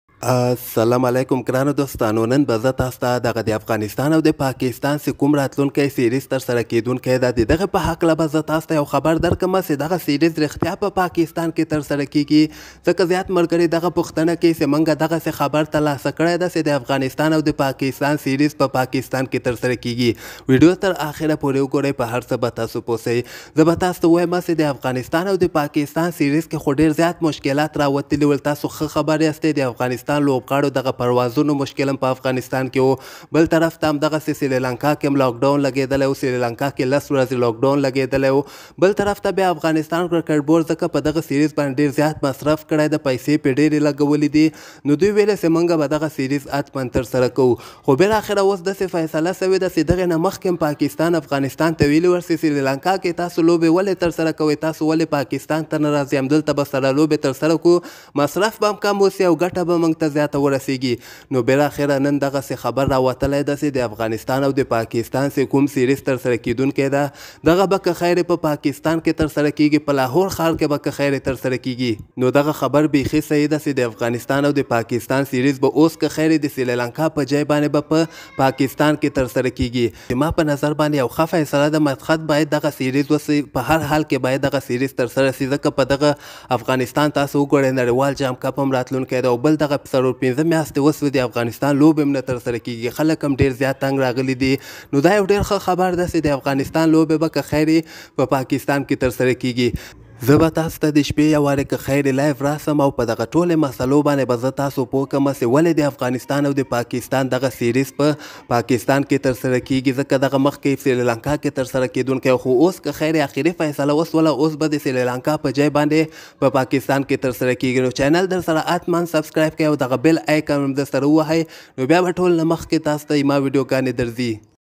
🔴Breaking News Afghanistan Vs Pakistan series will be in Pakistan not Srilanka
In this video I tell you about Breaking News Afghanistan Vs Pakistan series will be in Pakistan not Srilanka if you want to know more about it then watch complete video if you like it don't forget to subscribe.